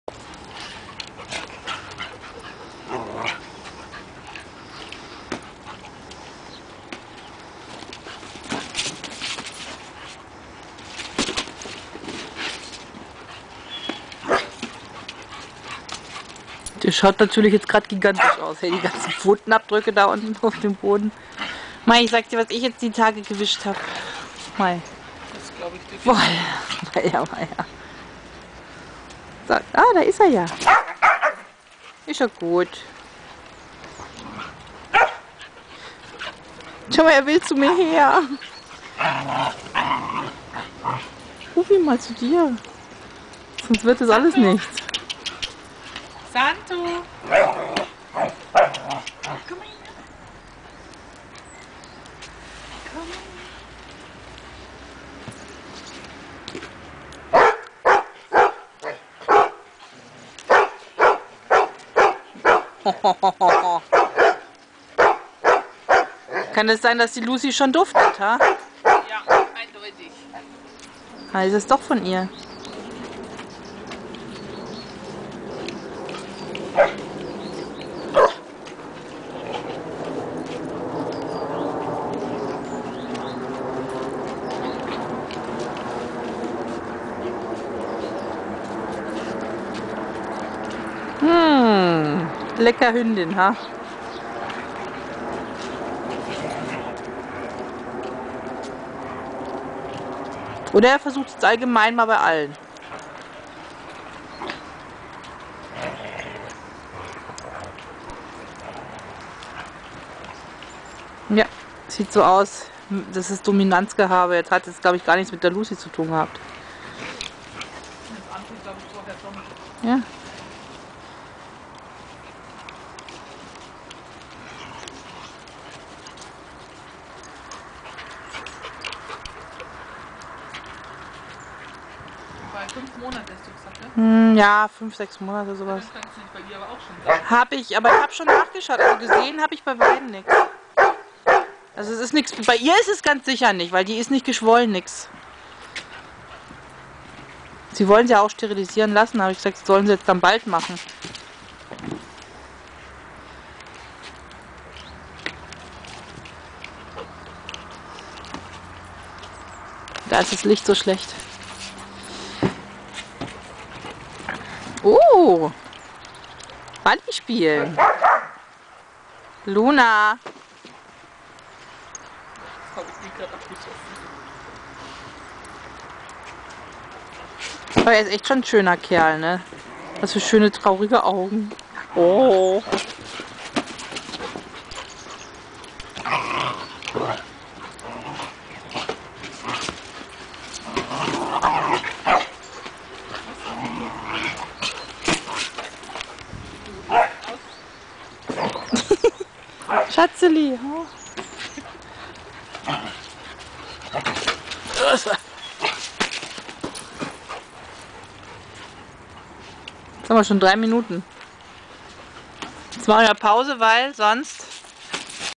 Snato hat jetzt ein Zuhause!!!
Santo der süße Grieche ist ca. 1 Jahr alt und wurde von vermittelt! Hier spielt er aber noch bei seiner Pflegestelle